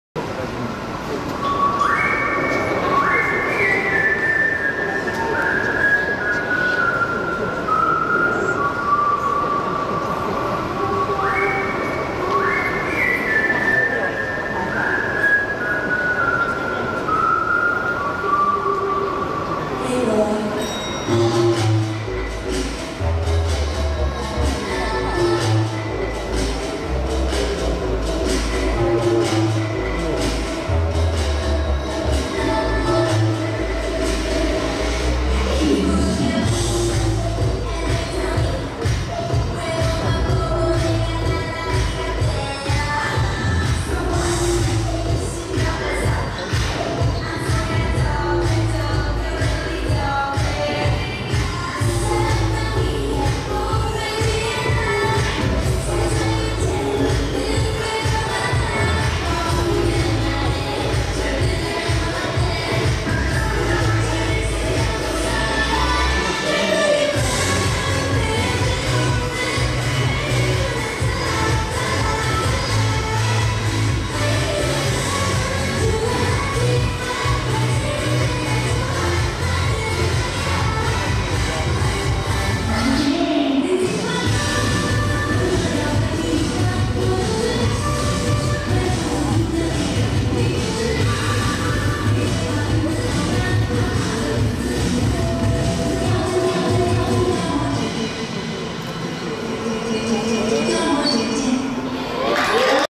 109.10.11 ~ 麗寶盃創意MV COVER 舞蹈比賽 ~ 大姐姐
麗寶百貨今年新增舉辦的MV COVER,分三次初賽取前六晉決賽~ 今日左右聯手出擊,隊名也變成大姐姐~覺得跳雙人舞很Q滿適合的,也順利晉級決賽~